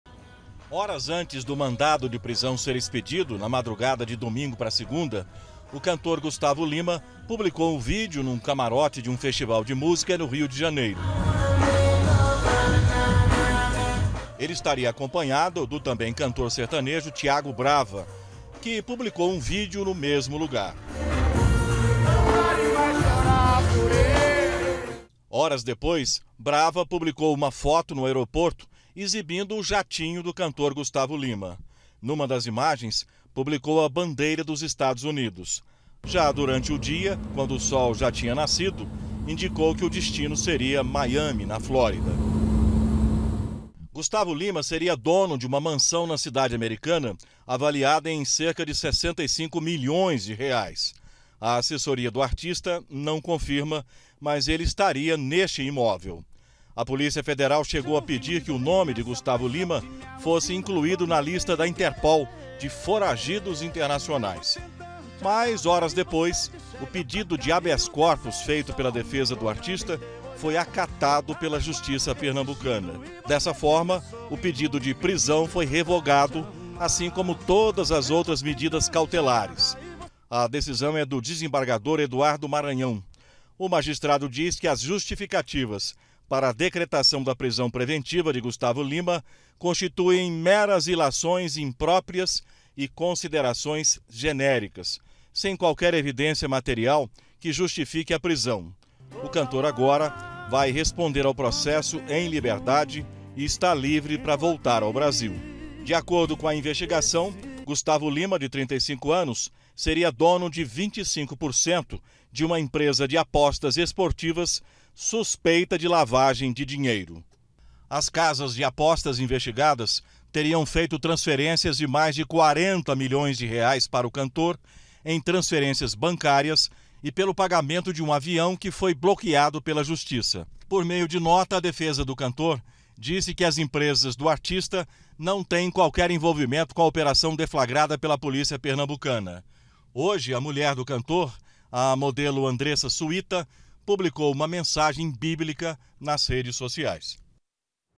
INVESTIGAÇÃO: JUSTIÇA REVOGA PRISÃO DO CANTOR GUSTTAVO LIMA
DETALHES DA REVOGAÇÃO DO PEDIDO DE PRISÃO DO CANTOR GUSTTAVO LIMA. A DECISÃO É DE UM DESEMBARGADOR DO TRIBUNAL DE JUSTIÇA DE PERNAMBUCO, E RELATOR DO CASO. ELE TAMBÉM SUSPENDEU A APREENSÃO DO PASSAPORTE E DA PERMISSÃO DO ARTISTA DE TER ARMA DE FOGO. GUSTTAVO LIMA TERIA NEGÓCIOS COM PESSOAS ENVOLVIDAS NUM ESQUEMA DE LAVAGEM DE DINHEIRO DE JOGOS DE AZAR E APOSTAS ESPORTIVAS.